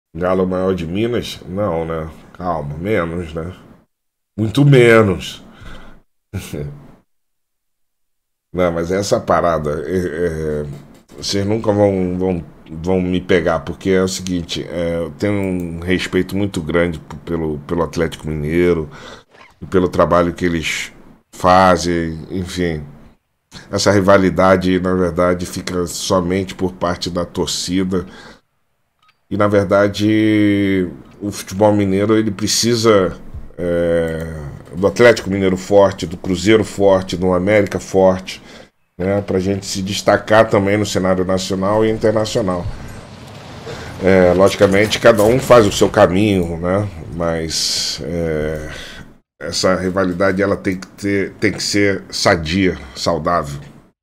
GALO MAIOR DE MINAS?! "CALMA, CALMA... MUITA CALMA!" | RonaldoTV
Ronaldo discute sobre a rivalidade entre Cruzeiro e Atletico Mineiro